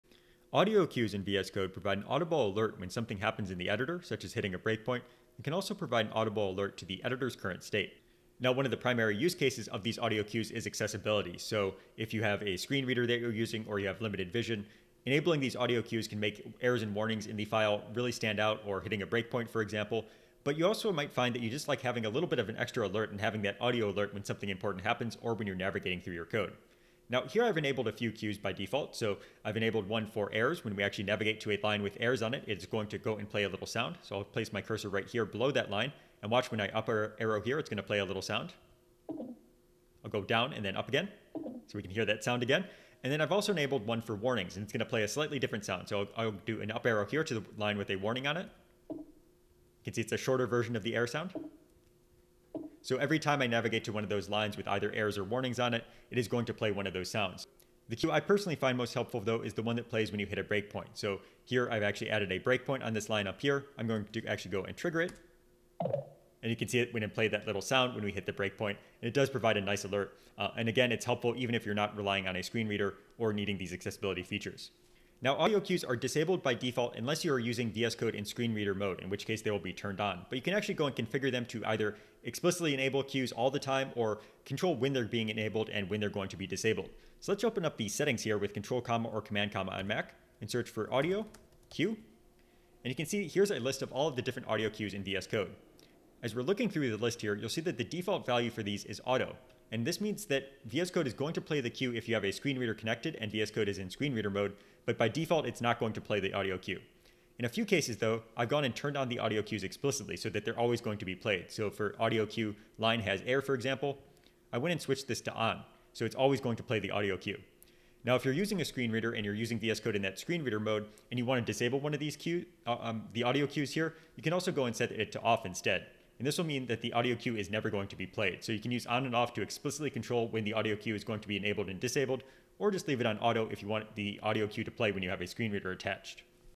VS Code tips — Audio cues
Audio cues in VS Code can provide audible alerts for events such as hitting a breakpoint and for when the current code line contains errors or is folded

They are on by default in screenreader mode. You can also explicitly enable/disable specific cues regardless of whether the editor is screenreader mode or not.